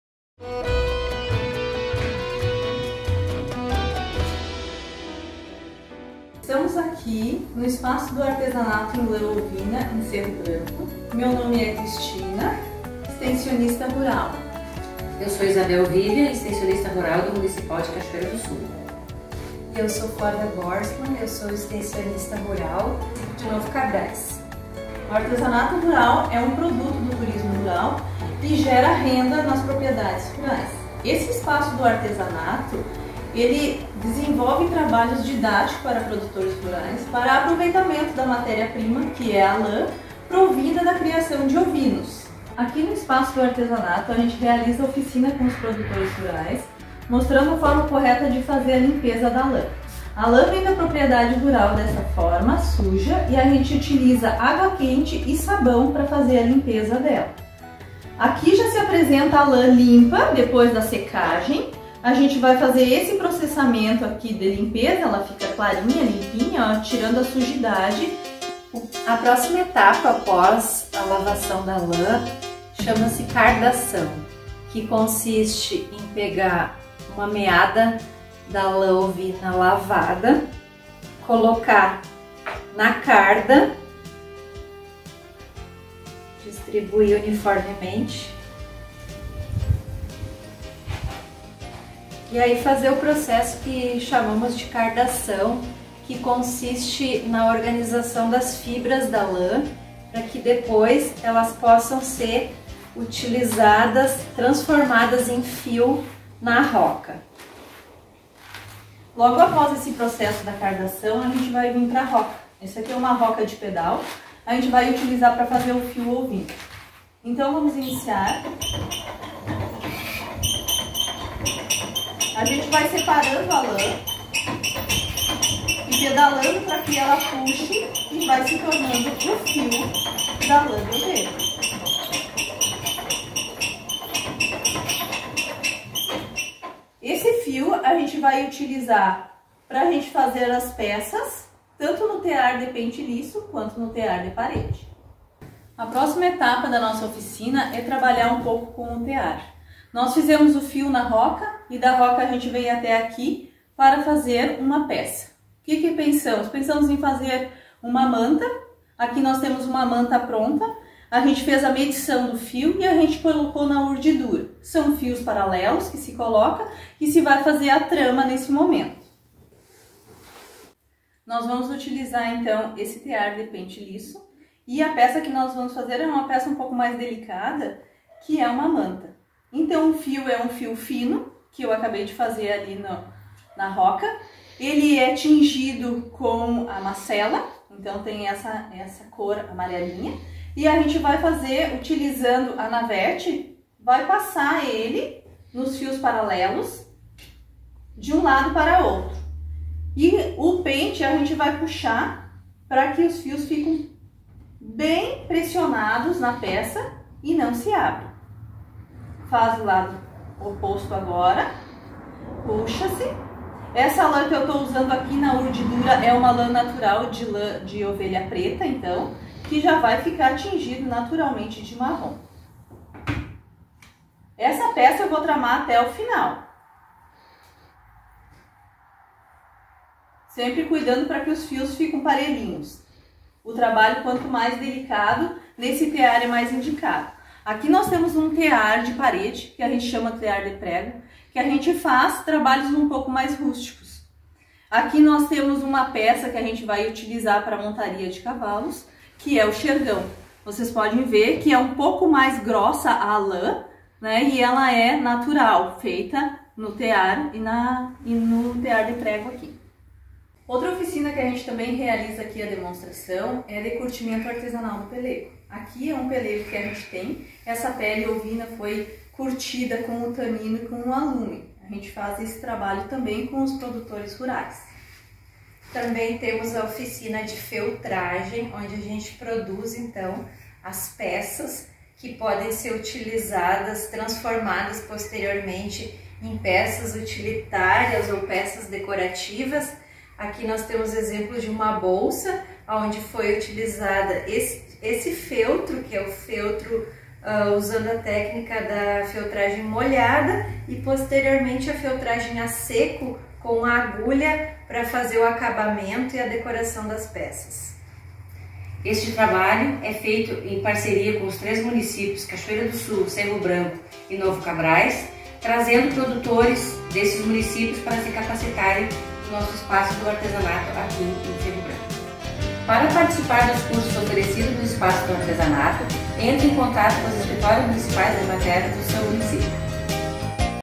Artesanato em lã ovina
O artesanato rural é um produto do turismo rural que gera renda nas propriedades rurais. No espaço do artesanato, extensionistas da Emater/RS-Ascar mostram como é realizado o aproveitamento da matéria prima (lã).